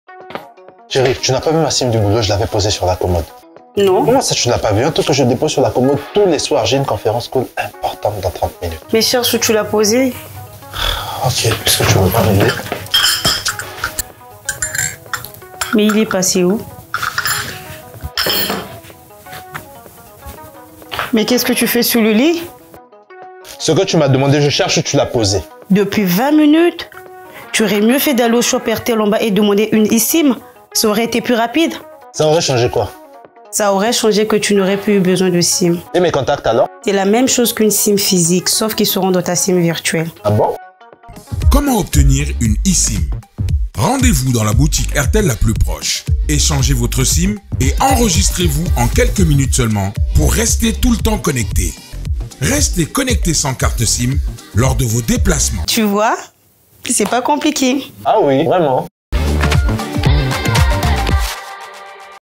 AIRTEL eSIM
Basculez dans la nouvelle ère de la communication avec l'eSIM Airtel🤳
Restez connectés sans carte sim partout où vous allez avec Airtel eSIM.
Composez *#06# pour vérifier votre compatibilité eSIM et l'EID.